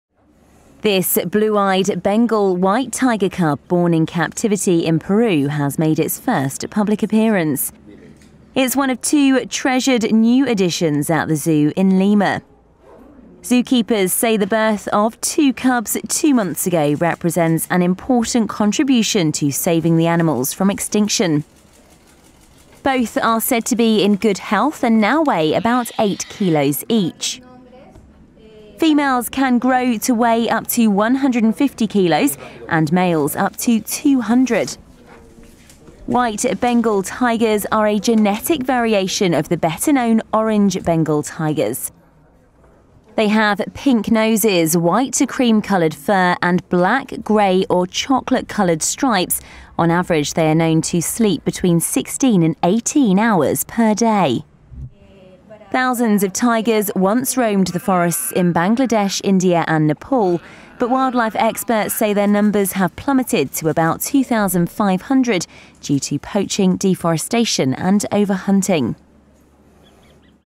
Two rare white Bengal tiger cubs make their debut
A Peruvian zoo shows off two treasured new additions, two rare Bengal white tiger cubs, one of the world's most endangered species.
Report by Sarah Duffy.